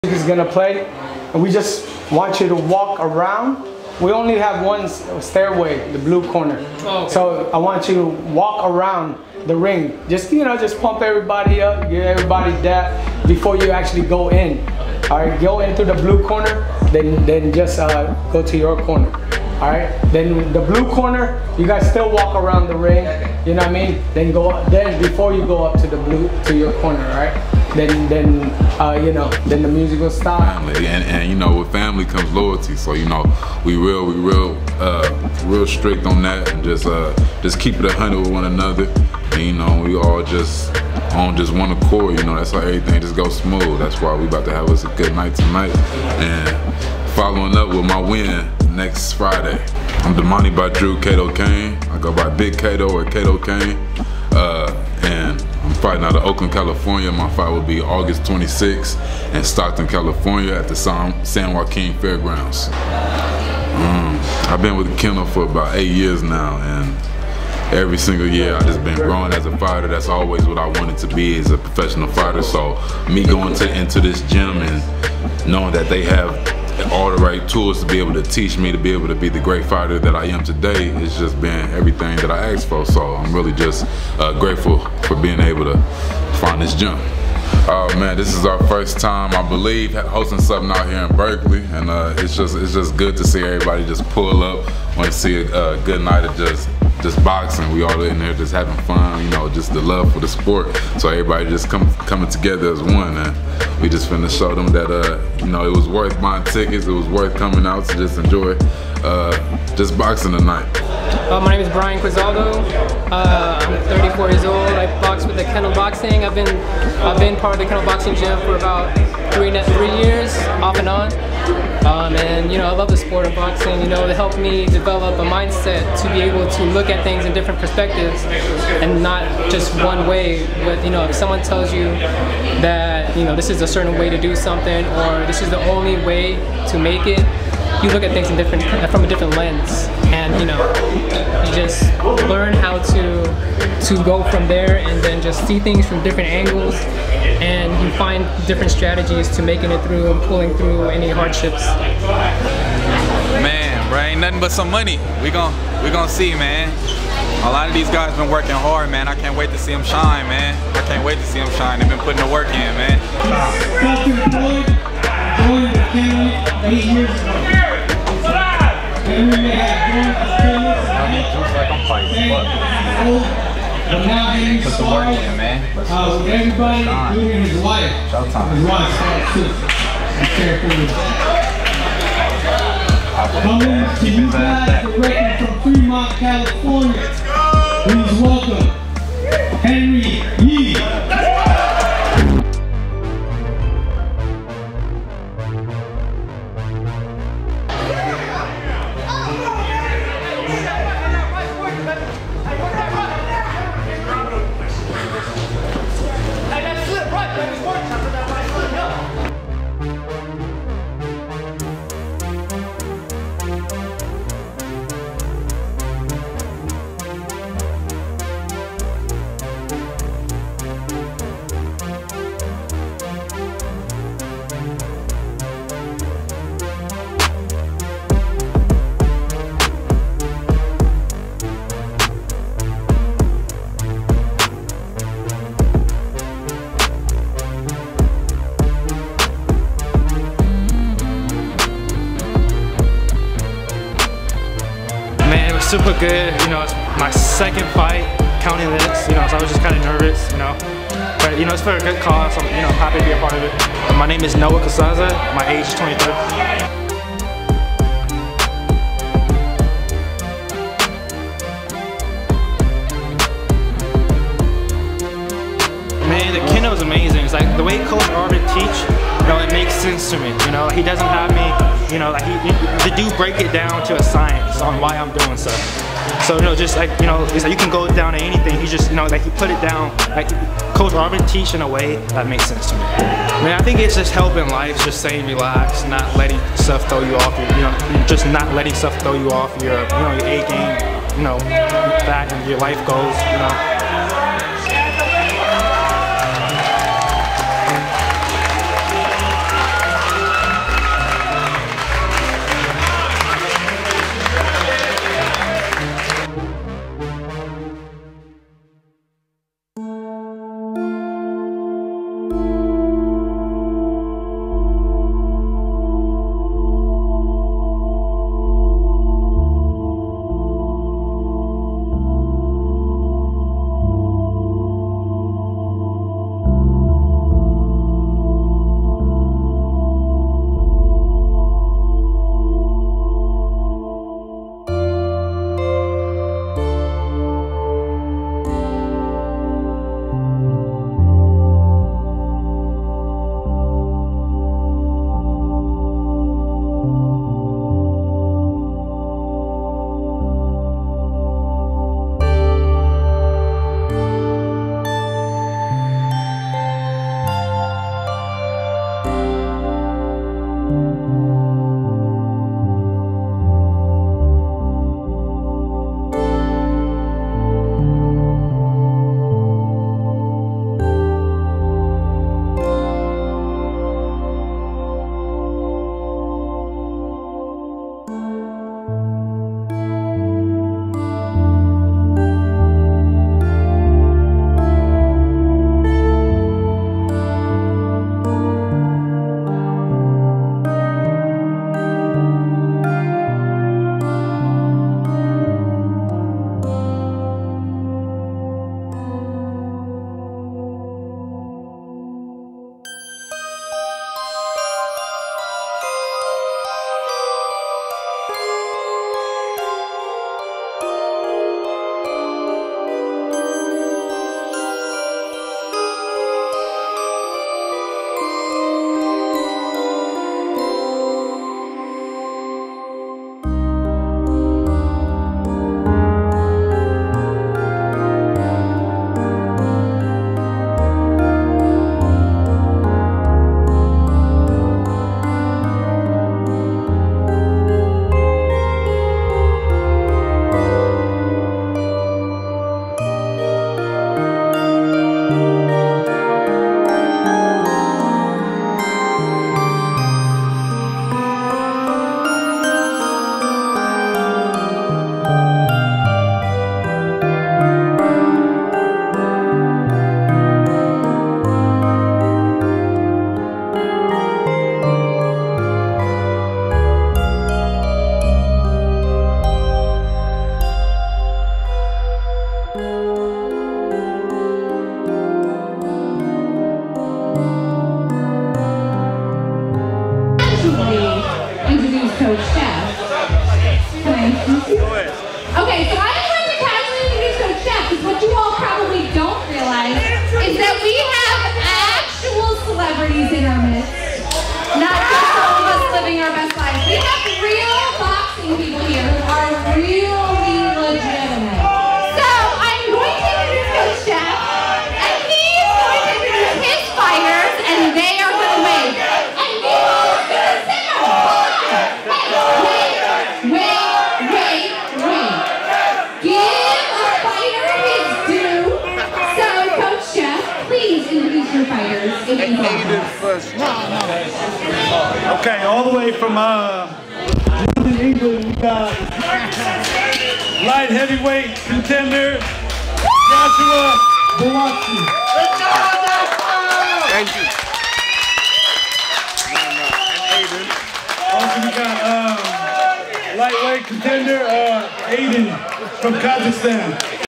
Kennel Boxing Gym Gets Support In Berkeley At Charity Event, Glenn Donaire Refs Bouts
This is a recap of a fundraiser held by The Kennel Boxing Gym in Berkeley, California, featuring Glenn Donaire, Joshua Bautsi, Damanoi Cato-Cain,